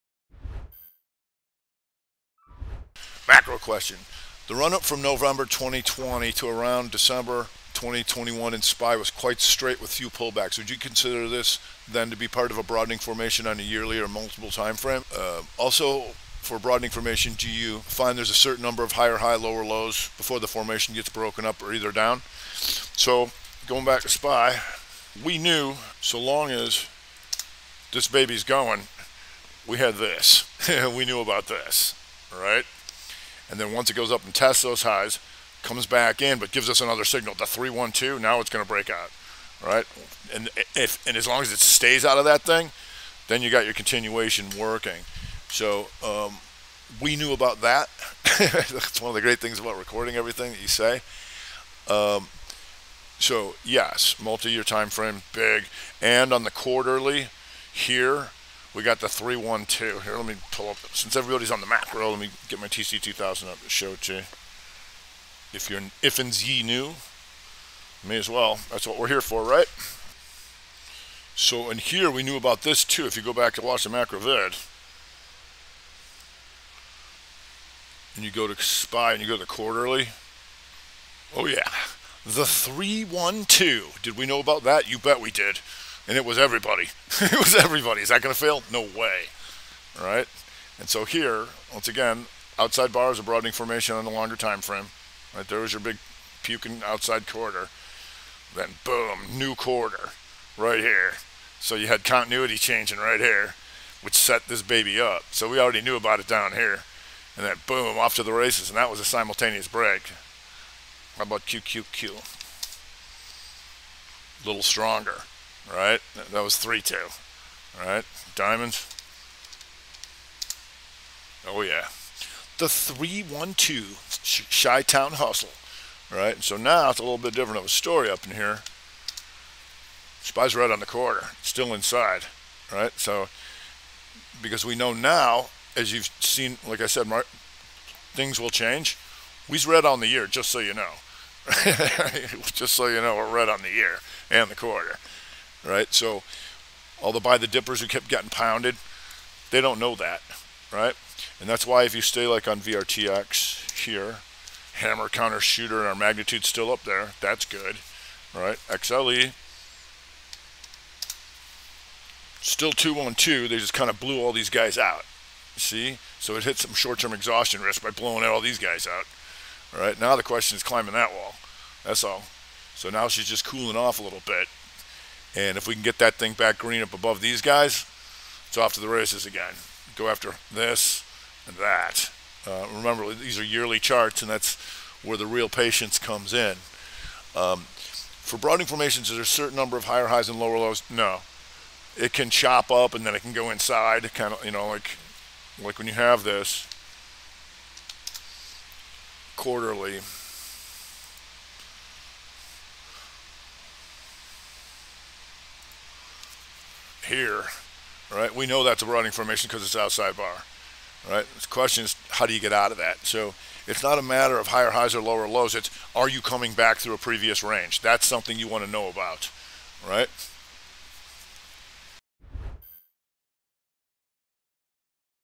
Rob Smith Power Hour - Broadening on the Yearly
Every Monday, Rob Smith answers your questions about TheSTRAT during The Power Hour in the live room. In this clip from April 25, Rob addresses a question about broadening formations on the yearly $SPY.